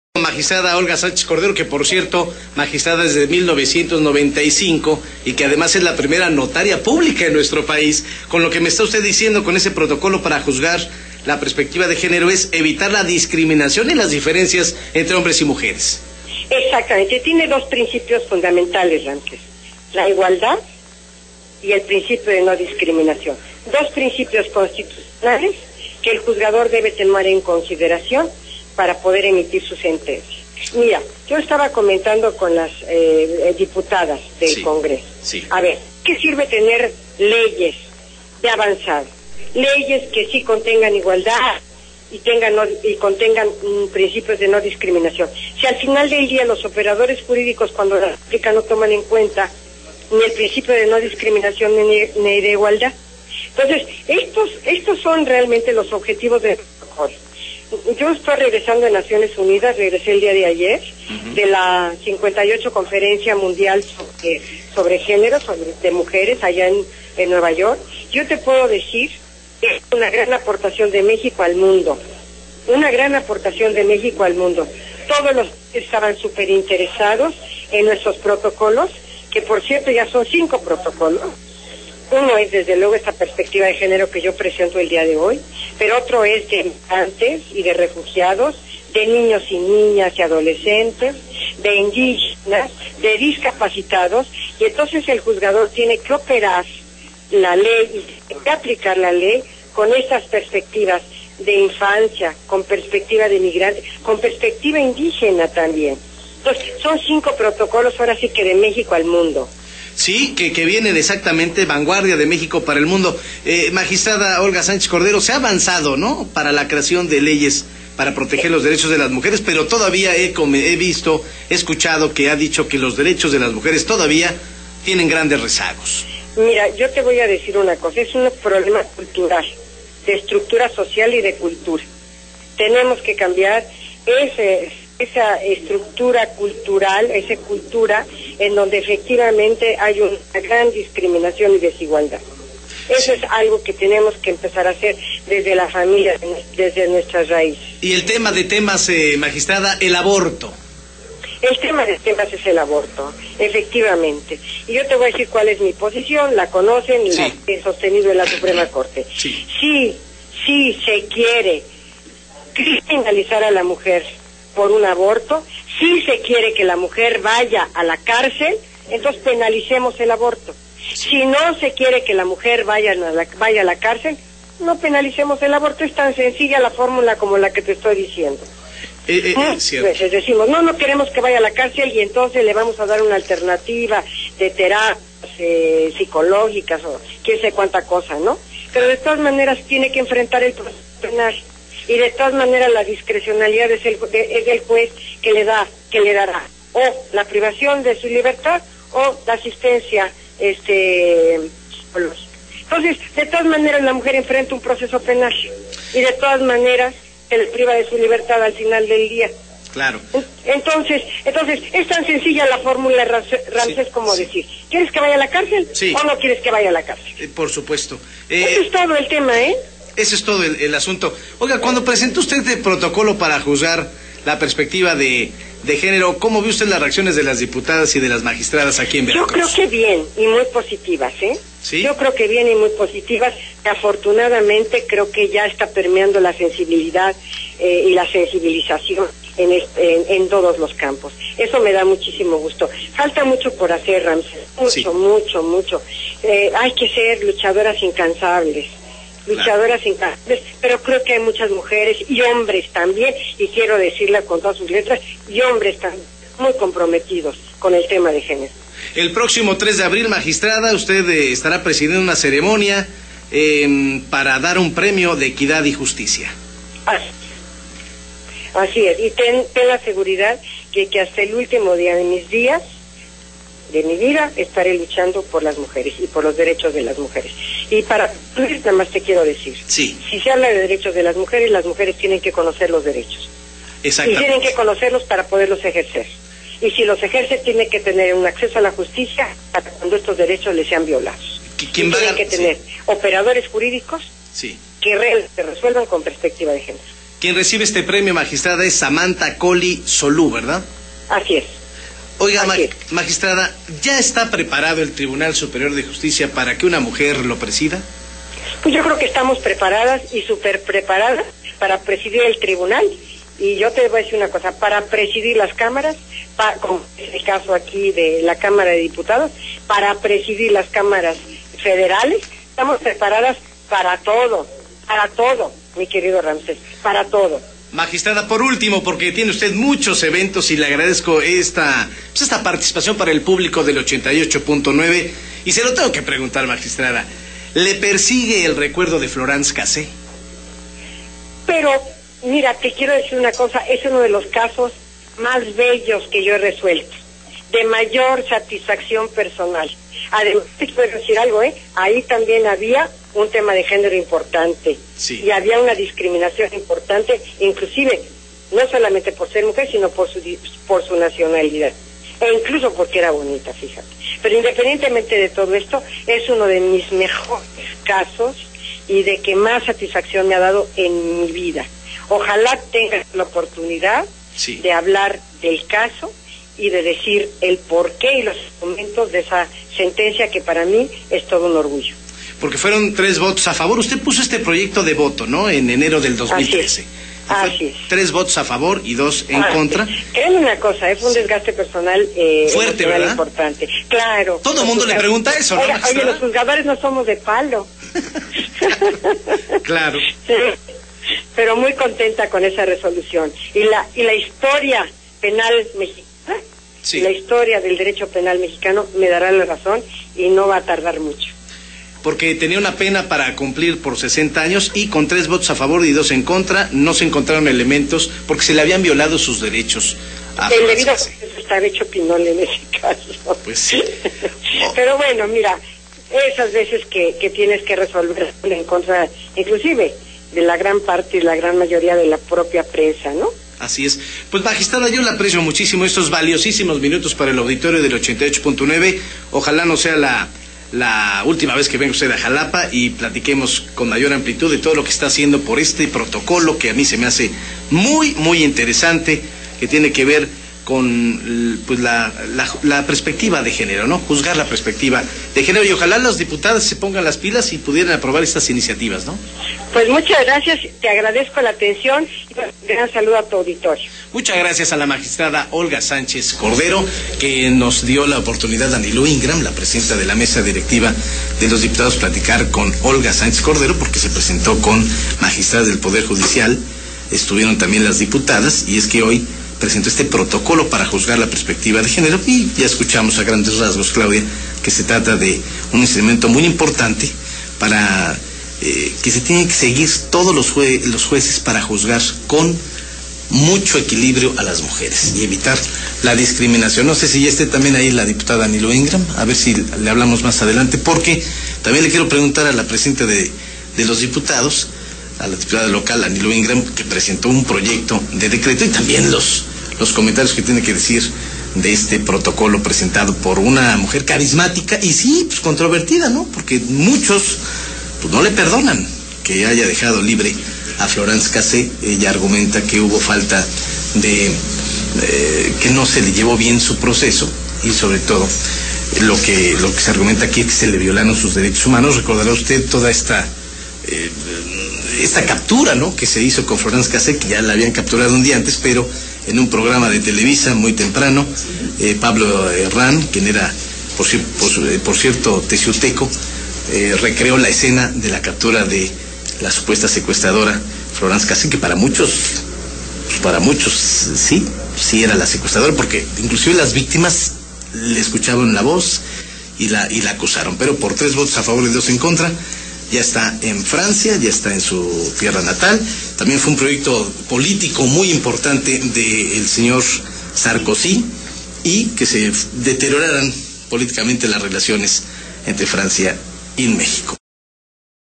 Uni Noticias 88.9 fm: Entrevista a Olga Sánchez Cordero
Entrevista realizada por el periodista Ramsés Yunes Zorrilla a Olga Sánchez Cordero, ministra de la Suprema Corte de Justicia de la Nación, con motivo de su visita a Veracruz, y la actualidad de la Corte (21/03/2014).